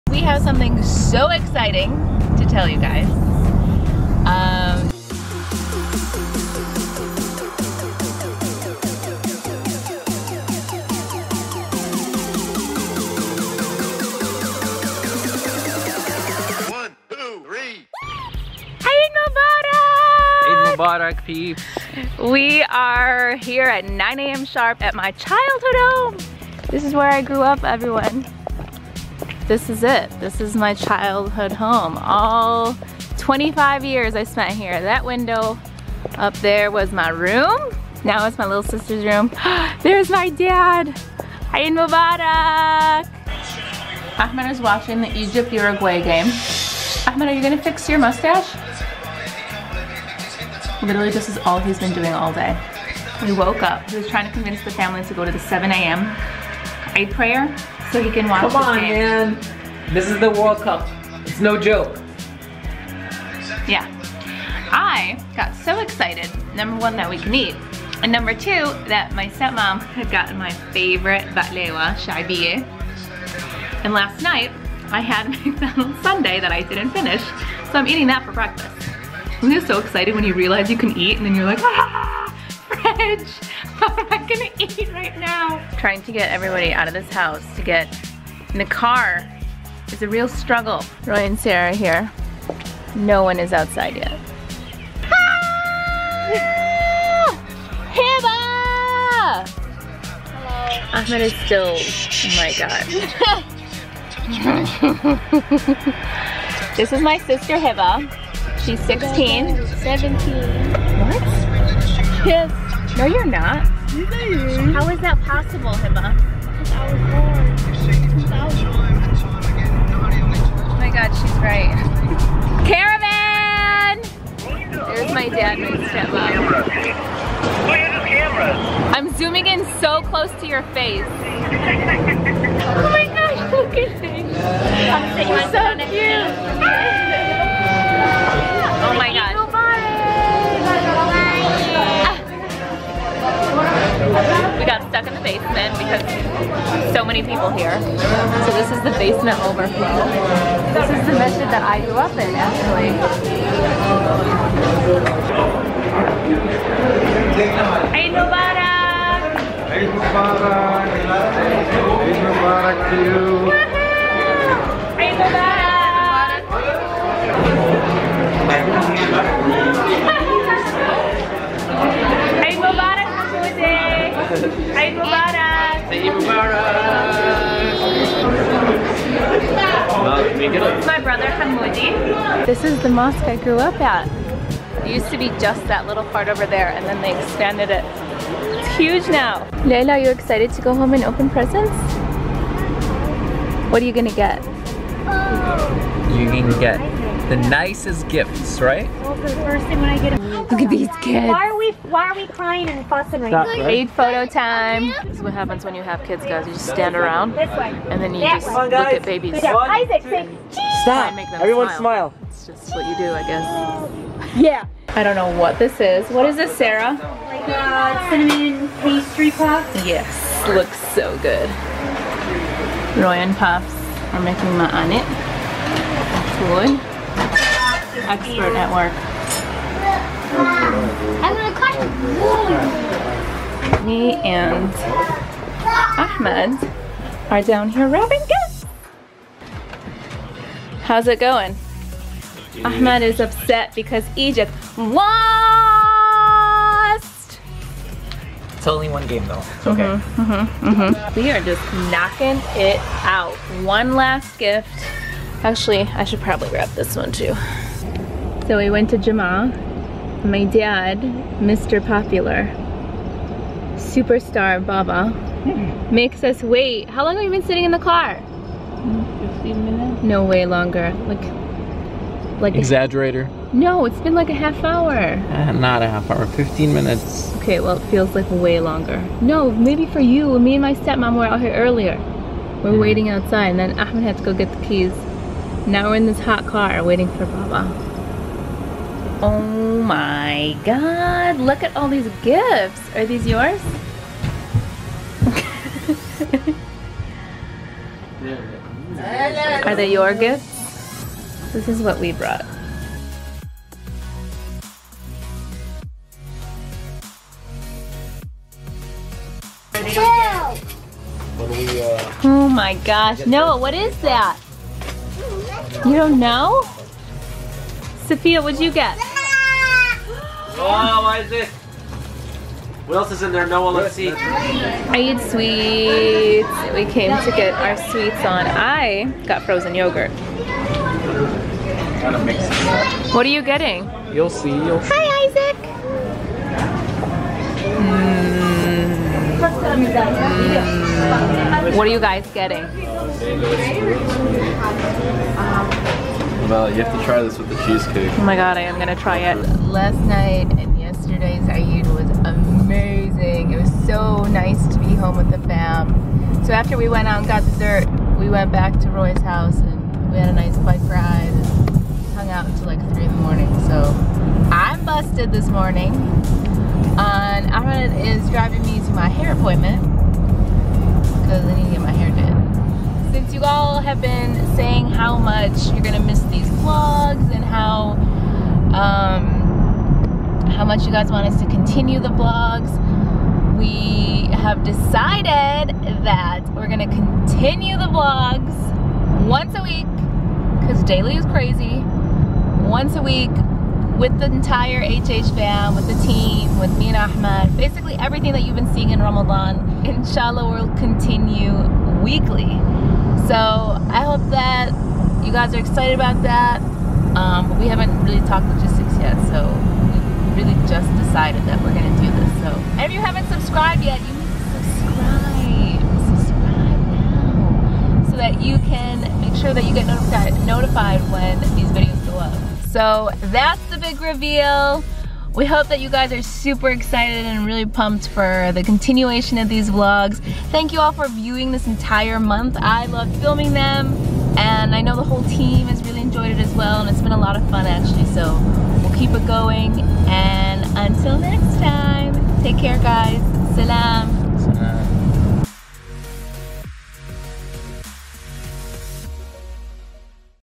EID VLOG + A BIG REVEAL
Haute Hijab Ramadan Diaries (Ep.31) - Unfortunately, Ramadan is over but at least you get to see how we spend Eid and we also are going to reveal an awesome surprise in todays vlog!

We teamed up with Betr Entertainment to make these daily Ramadan vlogs possible 💪🏽 Check them out + Instagram.com/betrofficial

{Join the HH Fam!}